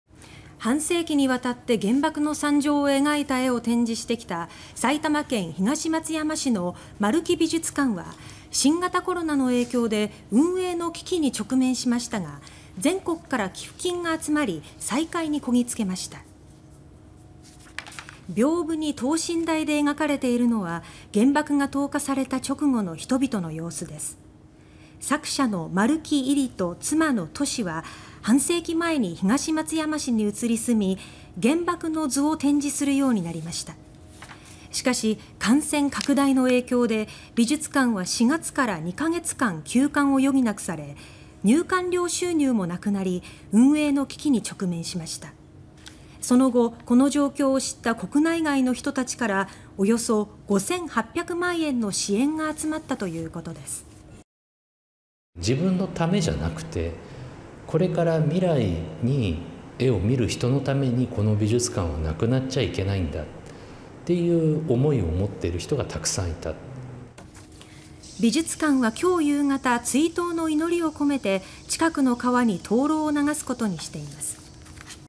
「原爆の図」美術館に運営危機 全国から寄付で再開(20/08/06)
半世紀にわたって原爆の惨状を描いた絵を展示してきた埼玉県東松山市の丸木美術館は新型コロナの影響で運営の危機に直面しましたが、全国から寄付金が集まって再開にこぎ着けました。

　屏風（びょうぶ）に等身大で描かれているのは原爆が投下された直後の人々の様子です。作者の丸木位里と妻の俊は半世紀前に東松山市に移り住み、原爆の図を展示するようになりました。しかし、感染拡大の影響で美術館は4月から2カ月間、休館を余儀なくされ、入館料収入もなくなって運営の危機に直面しました。その後、この状況を知った国内外の人たちから約5800万円の支援が集まったということです。
　丸木美術館学芸員・岡村幸宣さん：「（募金をした人のなかに）自分のためではなく、これから未来に絵を見る人のために、この美術館はなくなってはいけないという思いを持っている人がたくさんいた」
　美術館は6日夕方、追悼の祈りを込めて近くの川に灯篭（とうろう）を流すことにしています。